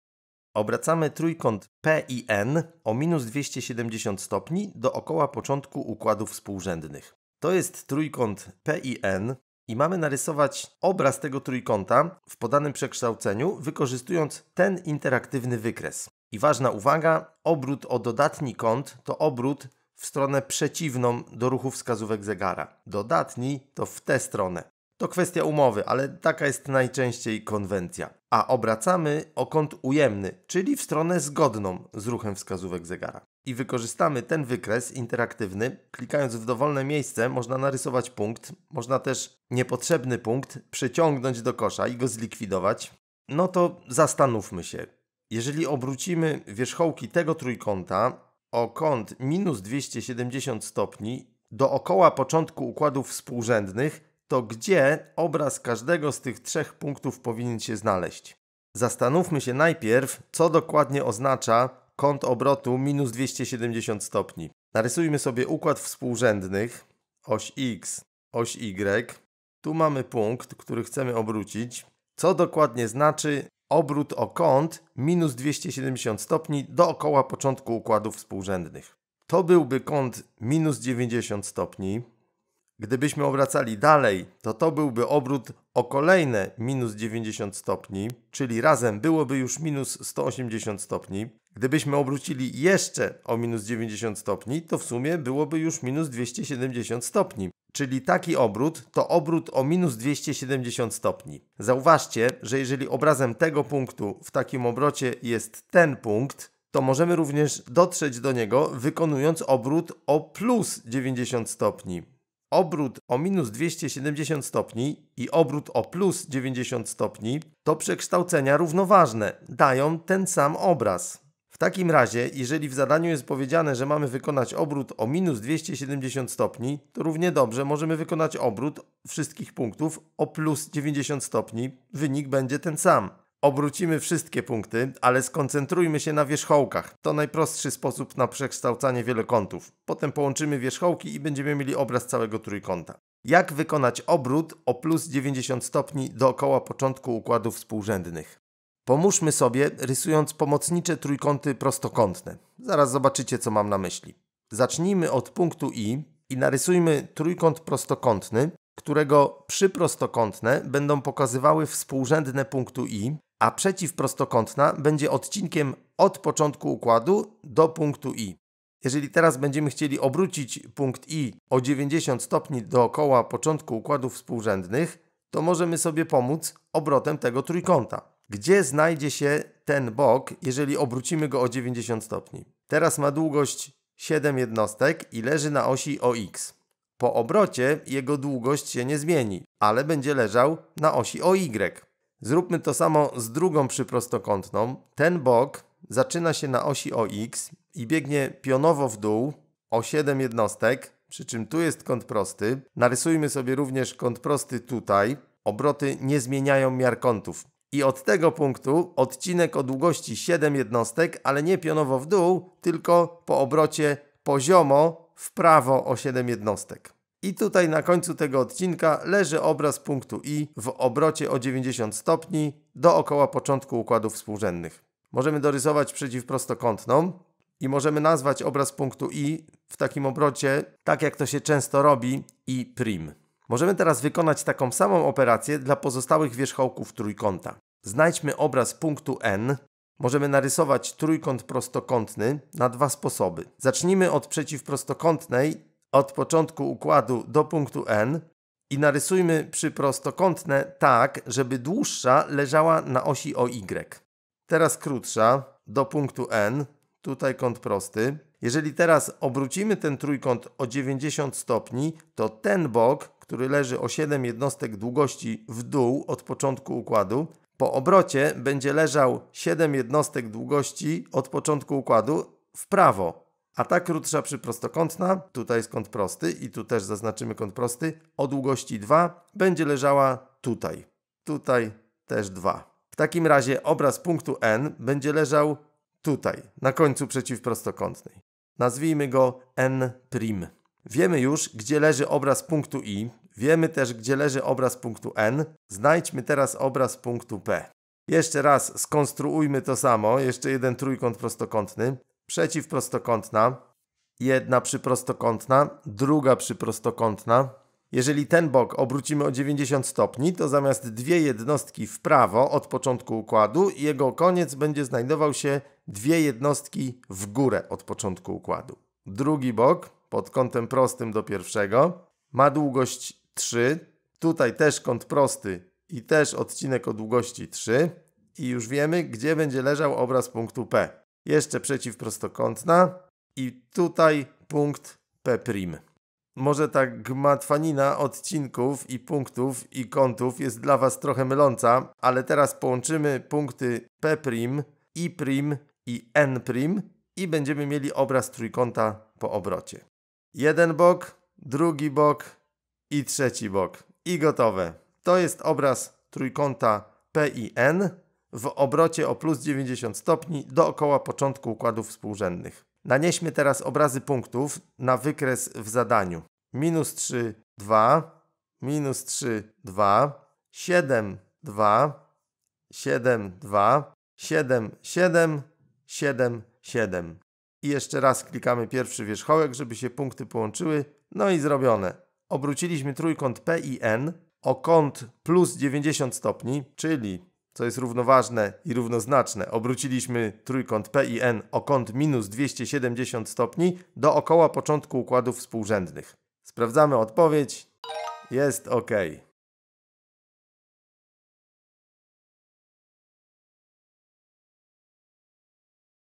Obrót wielokąta dookoła początku układu współrzędnych
Poćwicz znajdowanie obrazu wierzchołków wieloboku w obrocie dookoła początku układu współrzędnych